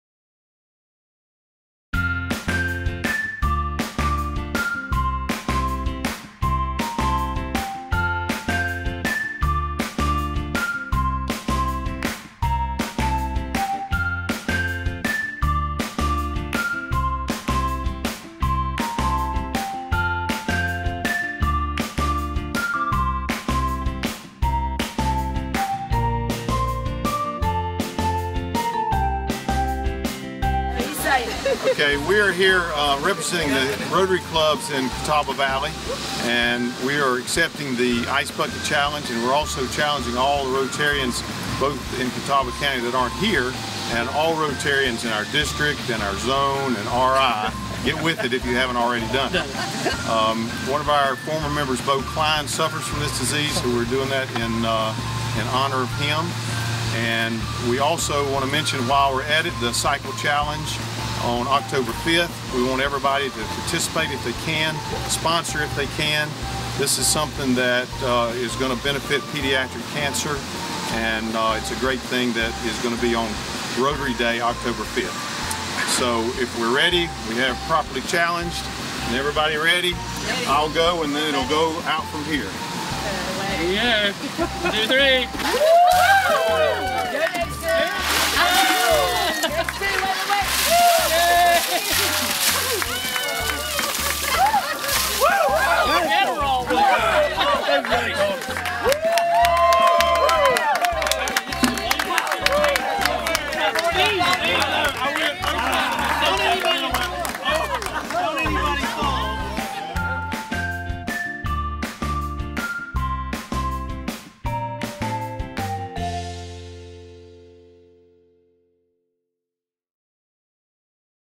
Hickory Rotary Club Ice Bucket Challenge!
The Hickory Rotary Club accepts the ALS Ice Bucket Challenge!